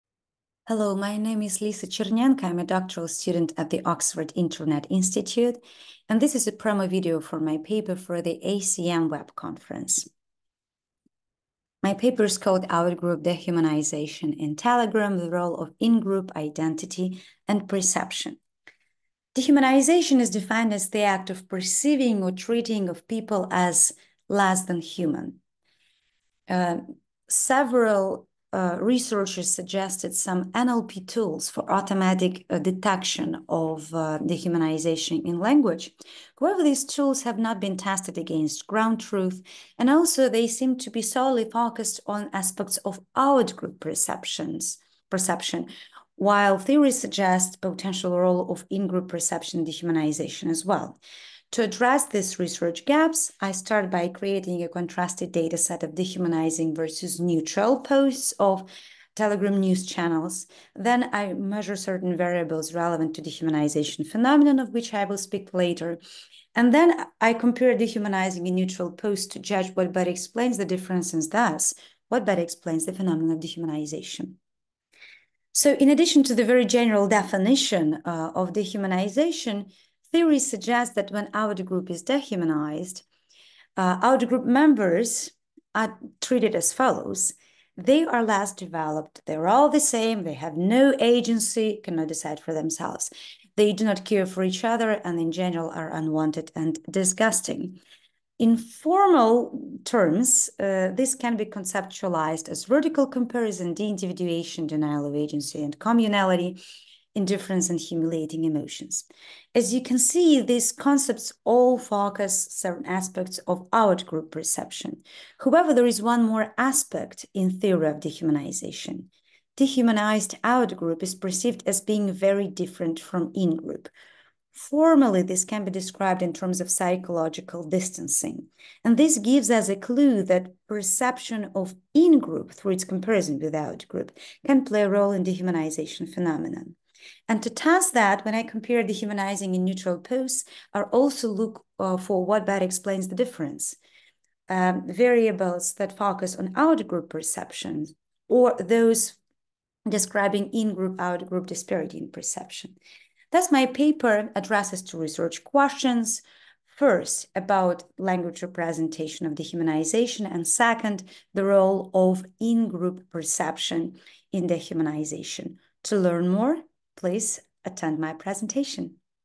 [phd4130] Outgroup dehumanisation in Telegram – the role of ingroup identity and perception
"Outgroup dehumanisation in Telegram – the role of ingroup identity and perception

Elizaveta Chernenko"